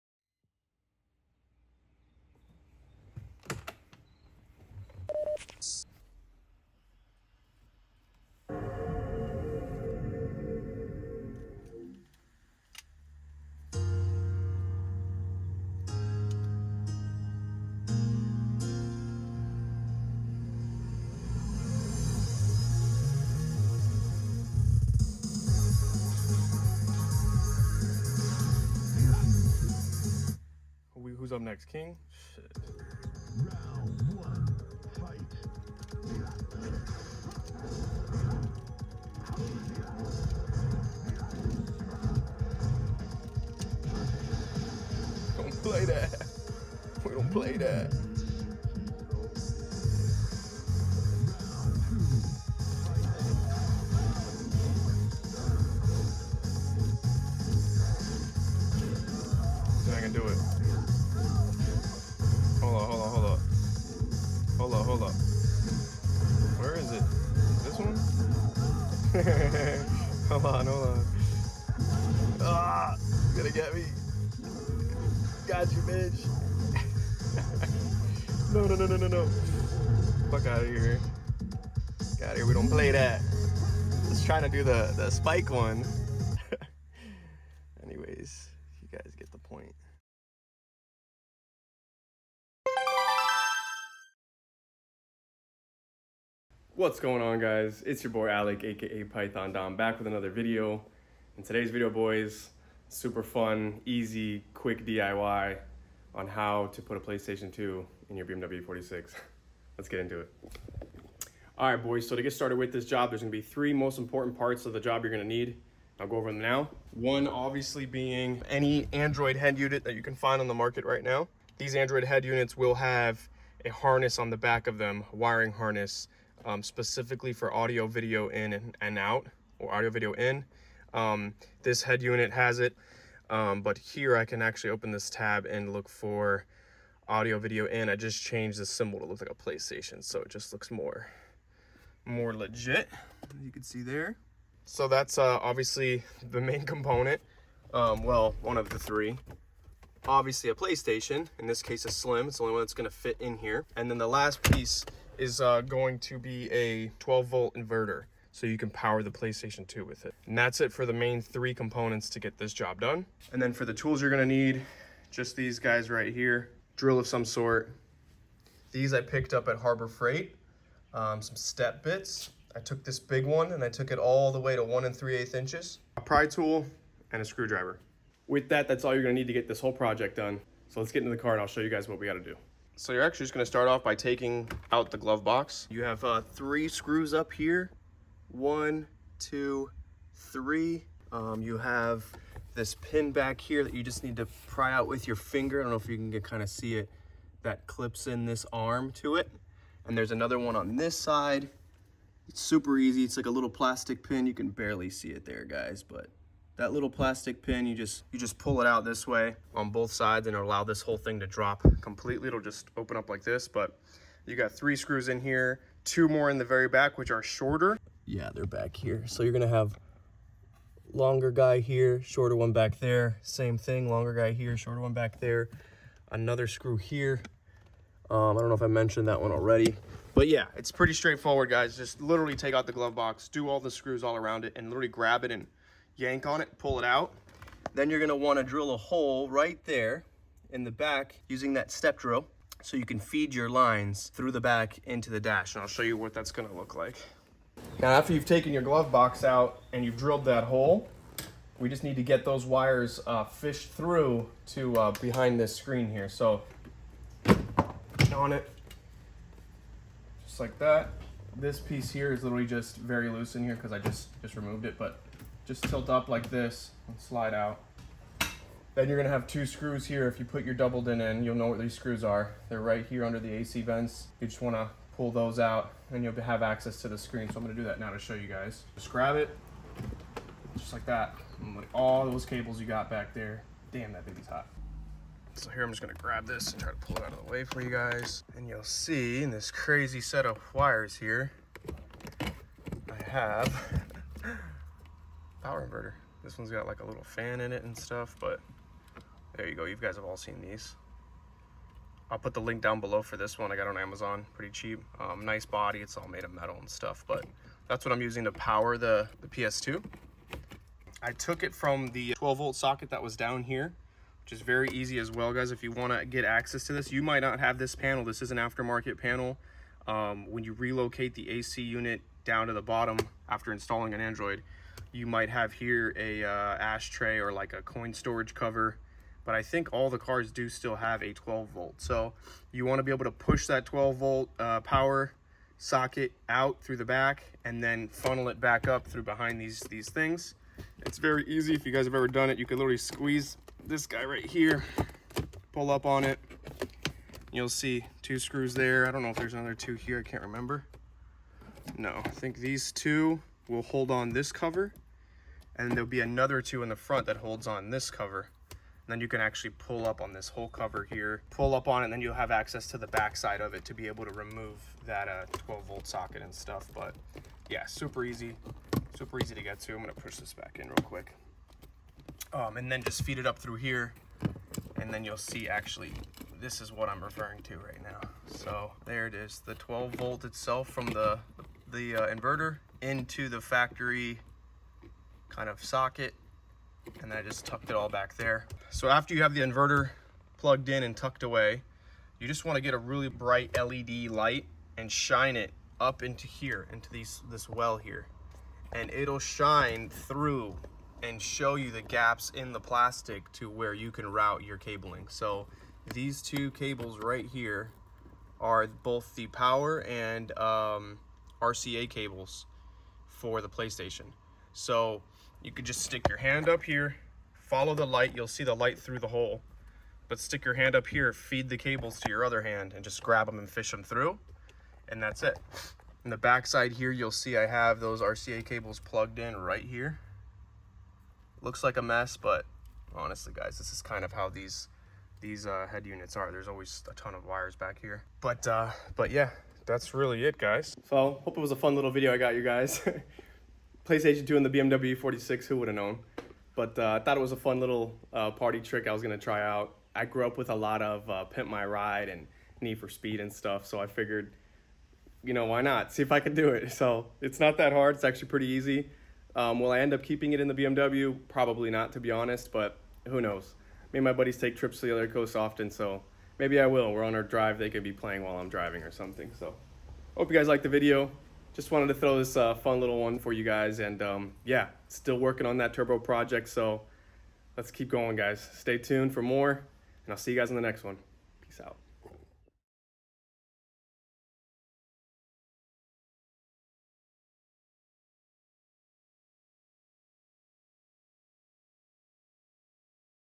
I Put a PS2 In my BMW E46!? - Easy DIY
LINKS TO ITEMS BELOW

AC RELOCATION BRACKET ---

Whats going on Guys!! Little break in between the turbo build! Just wanted to give you a quick DIY on how to put a PS2 in your BMW E46. Inspired by 2 Fast 2 Furious, Pimp my Ride, and just the early 2000s. Always wanted to try this and see if it was possible.